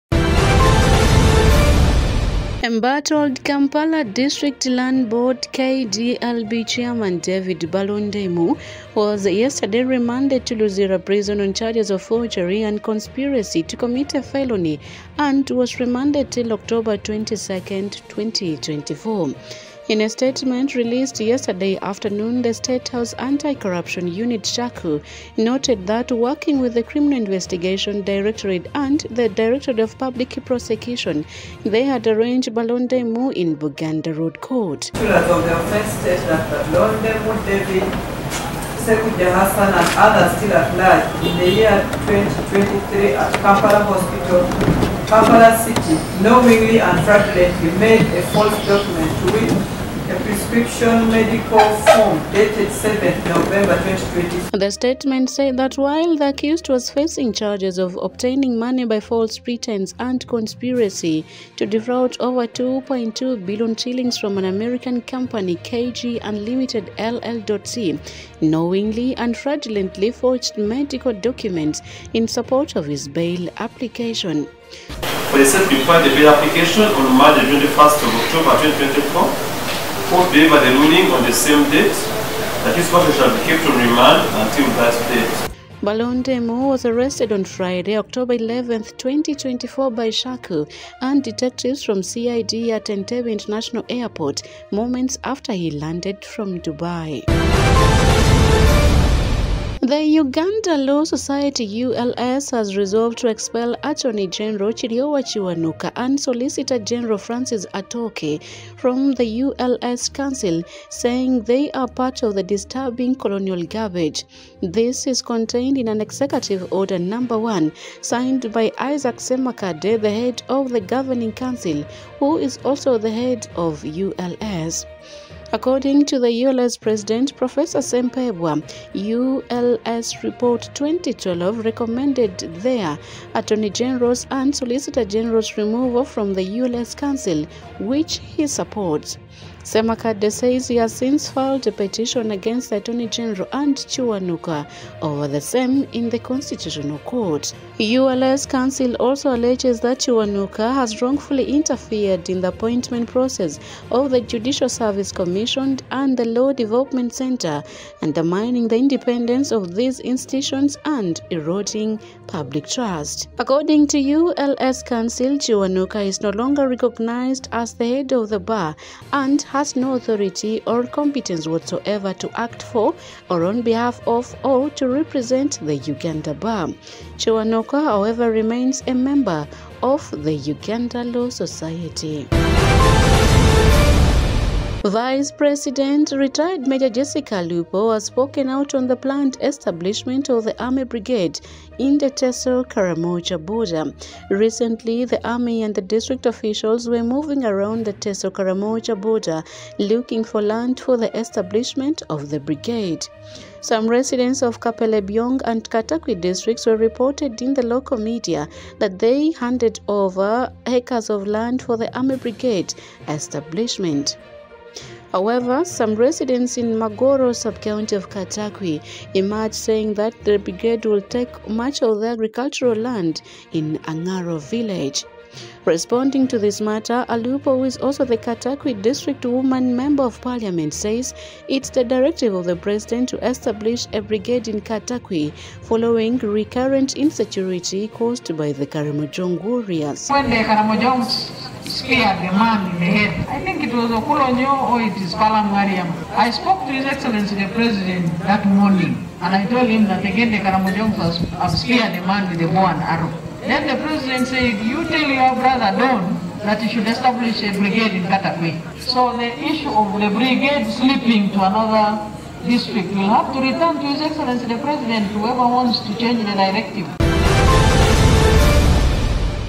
Morning DigitalDigest, 16.10.2024
‣ Balondemu remanded over forgery of medical papers
‣ Law society expels attorney general, solicitor general
‣ UPDF brigade set for Teso-Karamoja border